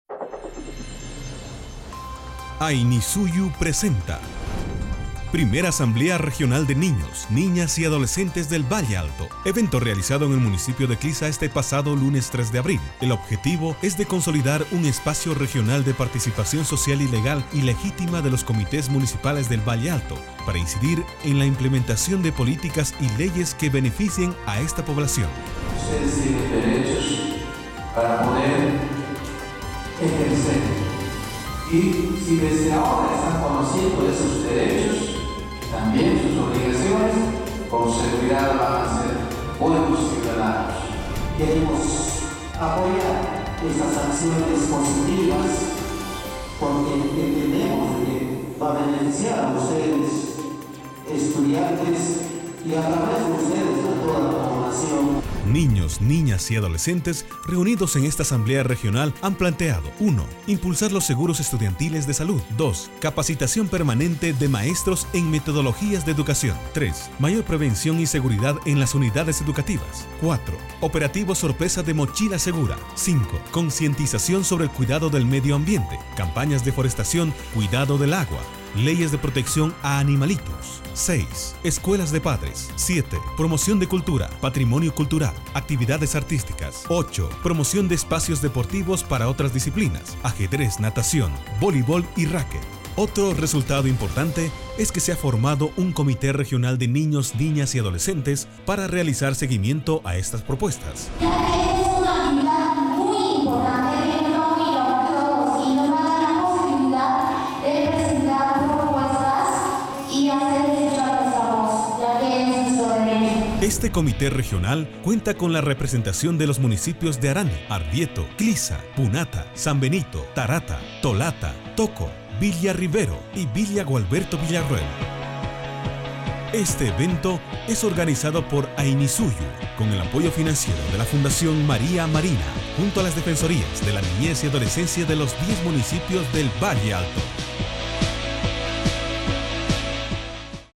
MICROINFORMATIVO PRIMERA ASAMBLEA DE NIÑOS, NIÑAS Y ADOLESCENTES DEL VALLE ALTO
AZC PRODUCCIONES